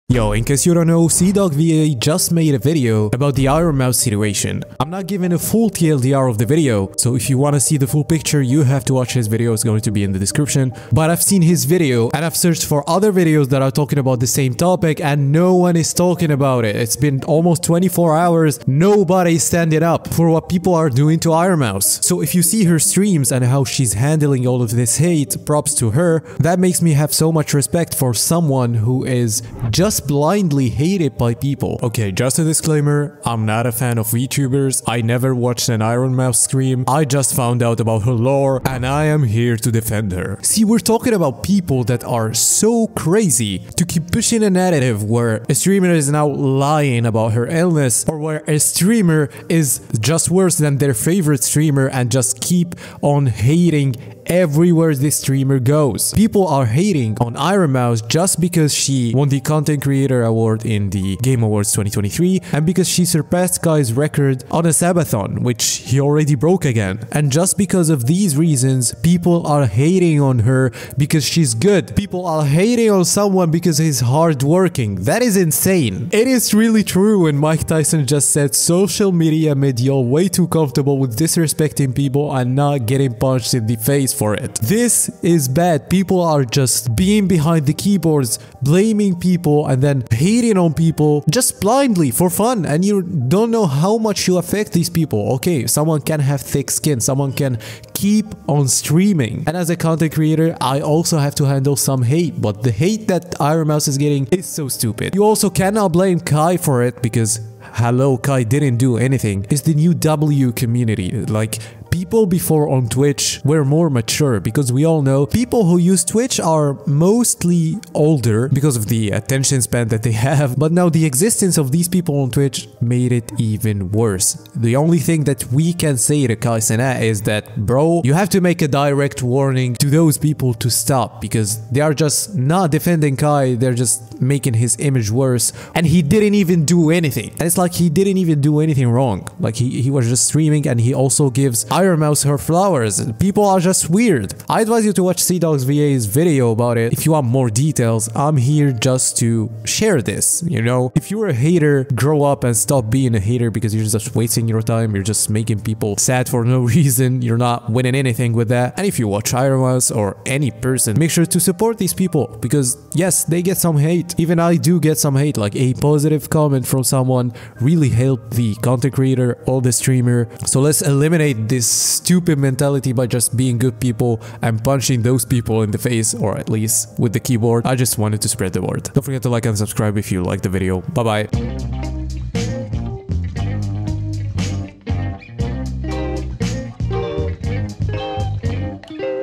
The IronMouse Situation Is Out Of Control !!!
Ironmouse has been getting too much undeserved hate lately and it needs to stop , people hate on someone that works hard is something only losers would do